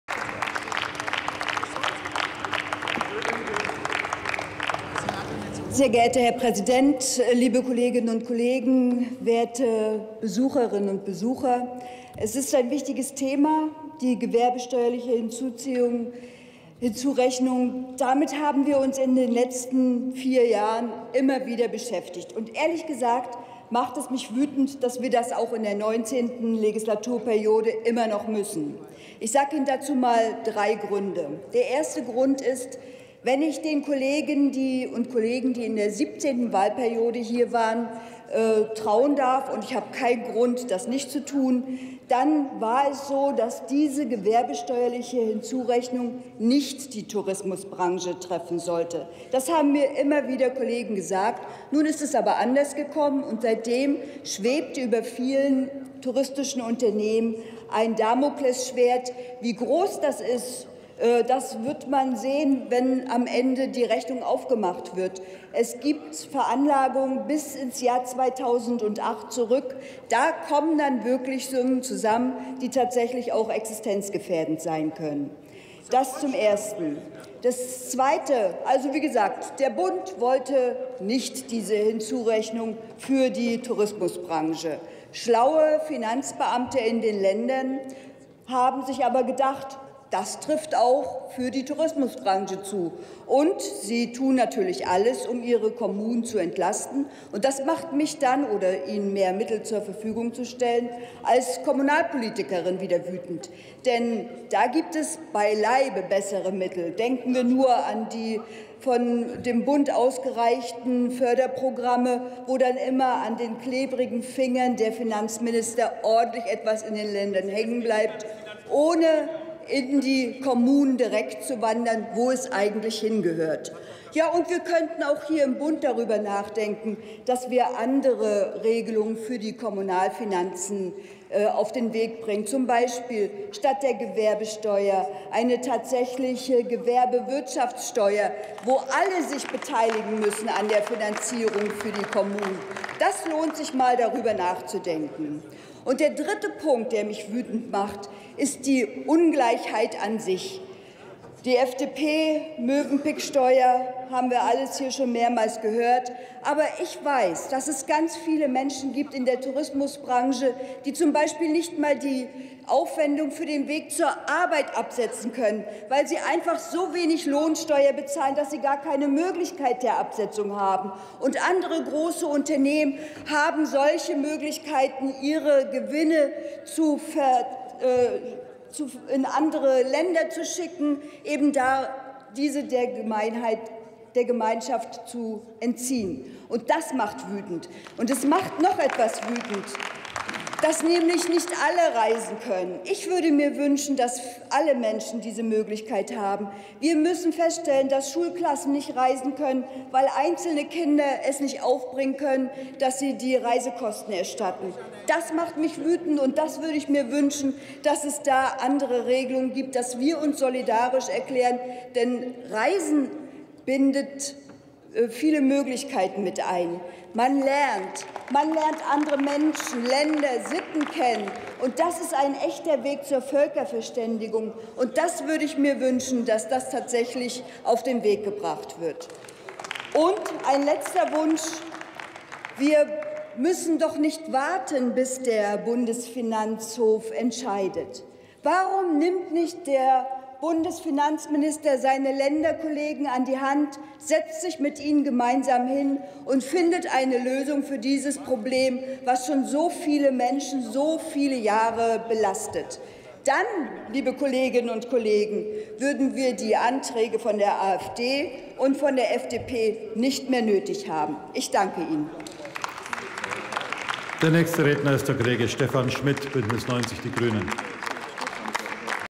Kerstin Kassner: Gewerbesteuerliche Hinzurechnung - Urlaubssteuer [Bundestag 29.06.2018]
Beratung des Antrags der Fraktion der AfD
Abschaffung der sogenannten Urlaubssteuer gemäß § 8 Nr. 1 Buchstabe e des Gewerbesteuergesetz – Vermeidung von Insolvenzen kleiner Reiseveranstalter
Drucksache 19/2989

ZP 12) Beratung des Antrags der Fraktion der FDP
Gewerbesteuerliche Hinzurechnung überprüfen und bei Missständen Abhilfe schaffen
Drucksache 19/2990

19. Deutscher Bundestag
43. Plenarsitzung
Tagesordnungspunkt: Gewerbesteuerliche Hinzurechnung - Urlaubssteuer
Redner: Kassner, Kerstin, Die Linke
Partei: Die Linke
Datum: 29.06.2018
Plenarprotokoll:
UVID:7249576